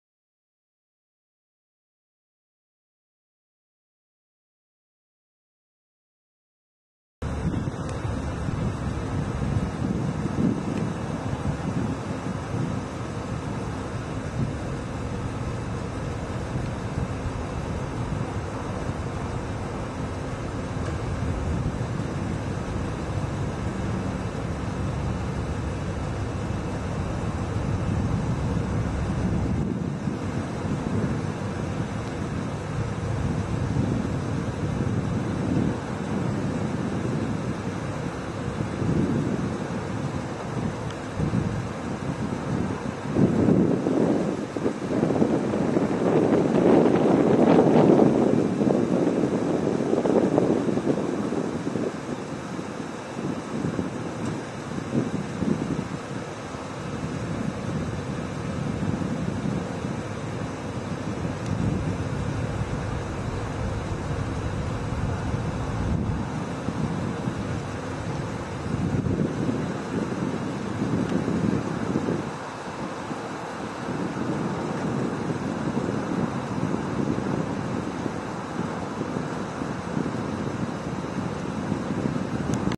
Passing Thermal
Waving flags change as thermal passes by